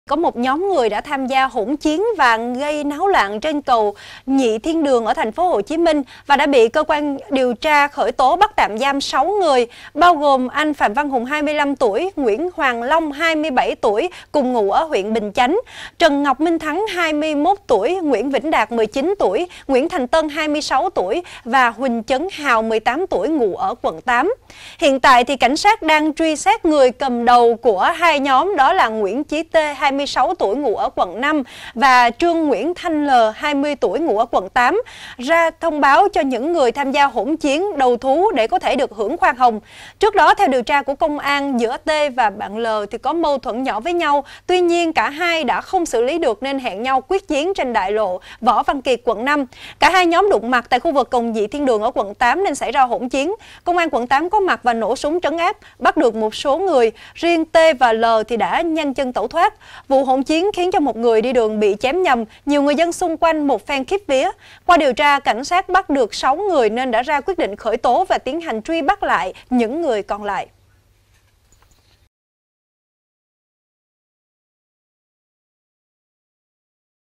Khởi tố 6 đối tượng tham gia hỗn chiến trên cầu Nhị Thiên Đường
VTC9 | Nhóm người tham gia hỗn chiến, gây náo loạn trên cầu Nhị Thiên Đường (TP.HCM) đã bị cơ quan điều tra khởi tố, bắt tạm giam.